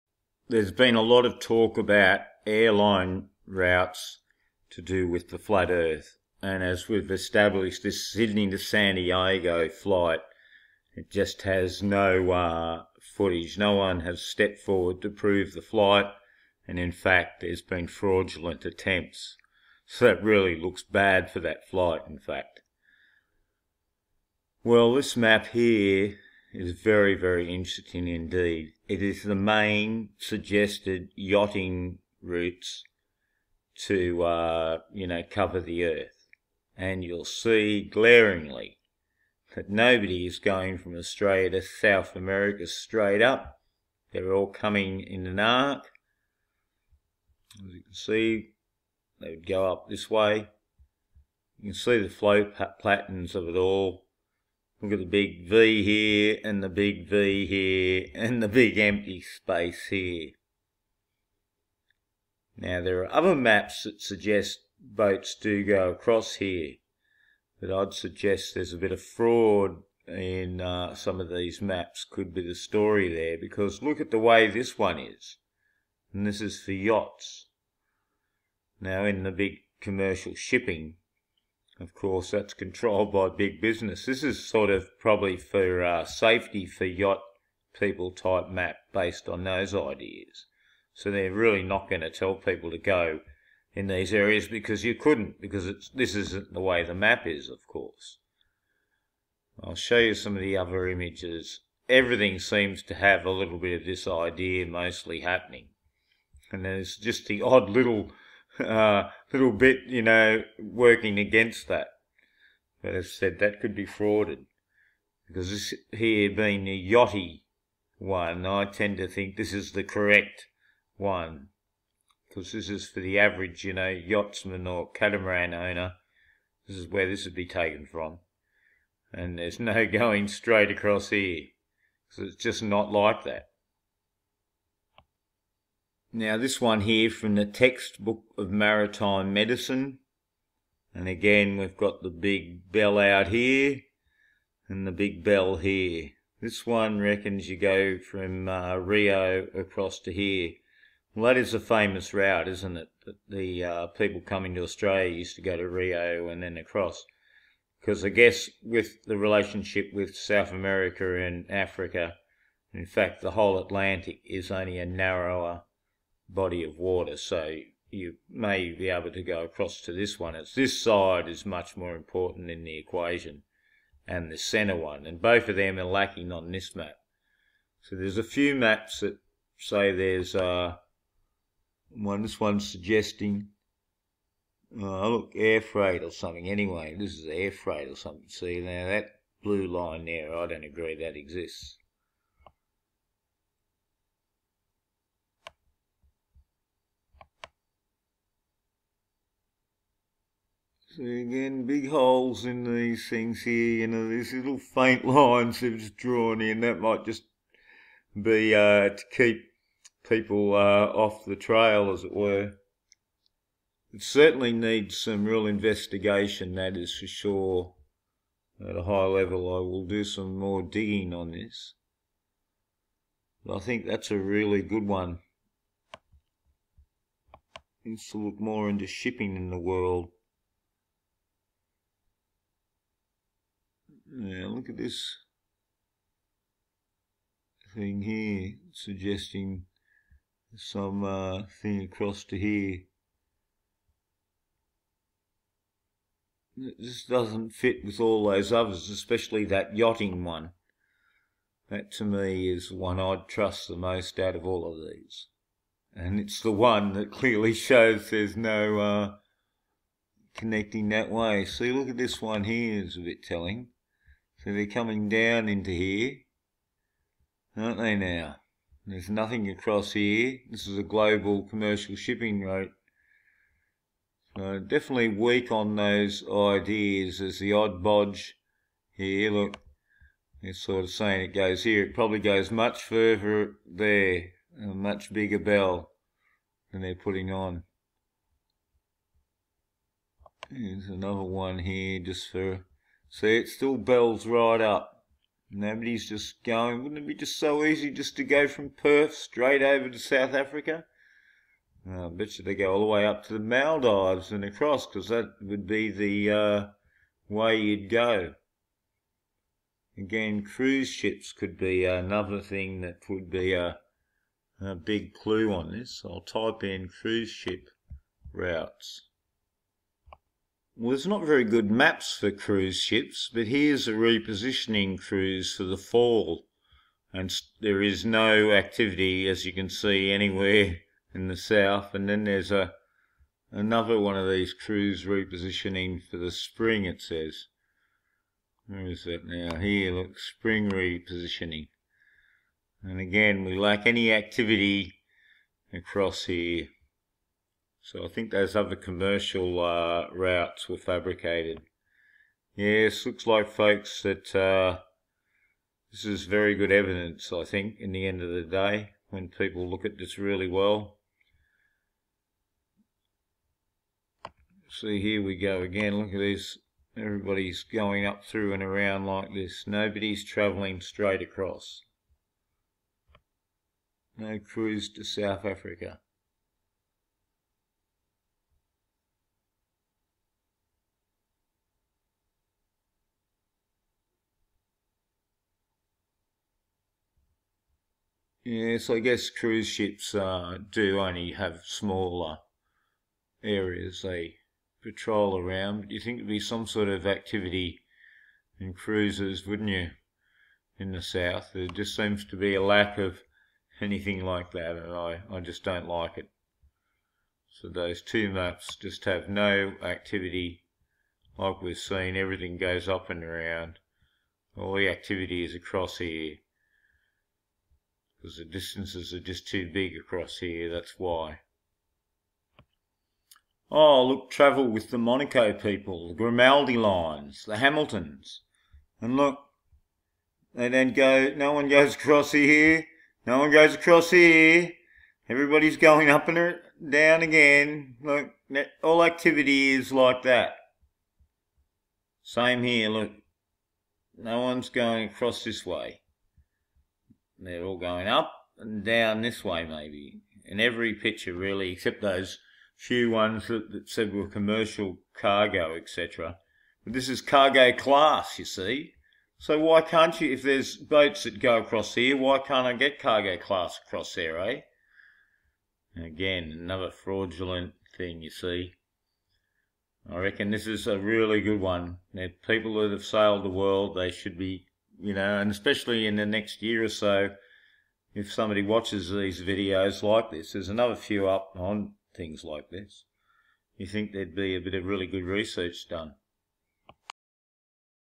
Flat earth look at Ship routes
Yes looks like sships dont like southern ocean seem to go nth then south The yatch map tells all